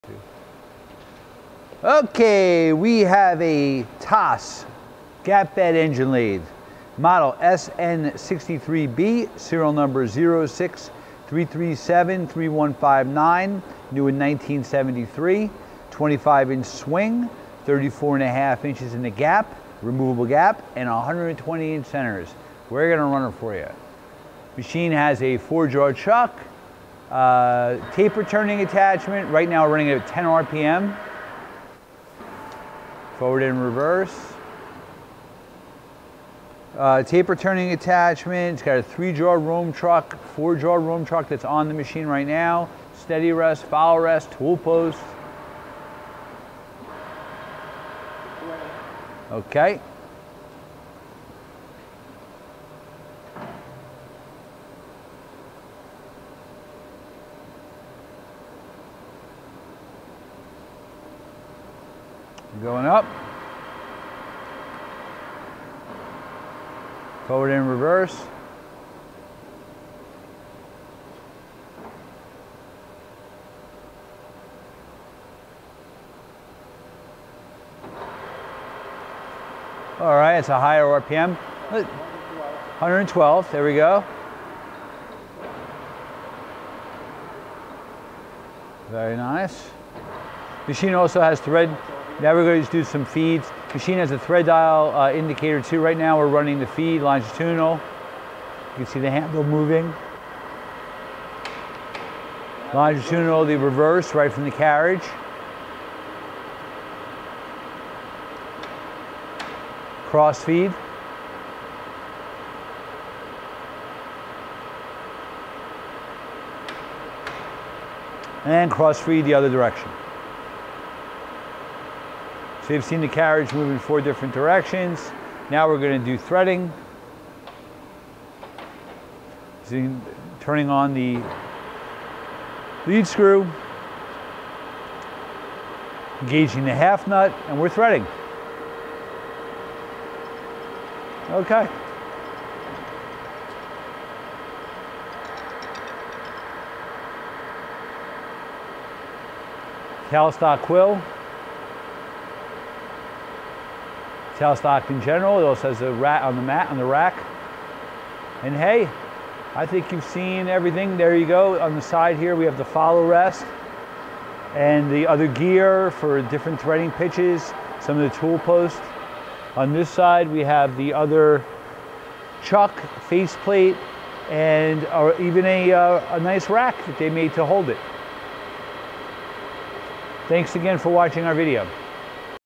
25"/34.5" X 120" TOS REMOVABLE GAP BED ENGINE LATHE
SPECIFICATIONS:
SWING OVER BED & CARRIAGE WINGS 25"
SWING OVER CROSS SLIDE 13.5"
SWING OVER GAP 34.5"
LENGTH OF GAP FROM SPINDLE NOSE 15.25"
DISTANCE BETWEEN CENTERS 120"
WIDTH OF BED 18"
SPINDLE SPEEDS (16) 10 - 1,000 R.P.M.
SPINDLE HOLE 2.75"
SPINDLE TAPER D1-8"
HEADSTOCK SPINDLE CENTER TAPER NO. 5 MORSE
FEED CHANGES (38):
LONGITUDINAL .002 - 0.242 IPR
CROSS .001 - 0.121 IPR
THREADS-INCH 1 TO 80 T.P.I.
THREADS-METRIC PITCH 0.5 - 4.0 MM
TAILSTOCK SPINDLE DIAMETER 3.5"
TAILSTOCK SPINDLE TRAVEL 9.5"
TAILSTOCK SPINDLE CENTER TAPER NO. 5 MORSE
APPROXIMATE DIMENSIONS 200" x 60" x 62"

EQUIPPED WITH:
- REMOVABLE GAP BED
- HARDENED AND GROUND BEDWAYS
- QUICK CHANGE GEARBOX WITH INCH/METRIC THREADING CAPABILITY
- TAPER TURNING ATTACHMENT
- 3-JAW ROHM CHUCK
- 4-JAW ROHM CHUCK
- FACEPLATE
- FOLLOW REST
- 12" DIAMETER CAPACITY THREE POINT ROLLER-TYPE STEADY REST
- QUICK CHANGE TOOLPOST WITH TWO HOLDERS
- MICROMETER CARRIAGE STOP
- THREADING DIAL
- REMOVABLE CHIP PAN
- 1 HORSE POWER RAPID TRAVERSE MOTOR
- 10 HORSE POWER MAIN DRIVE MOTOR AND ELECTRICAL CONTROLS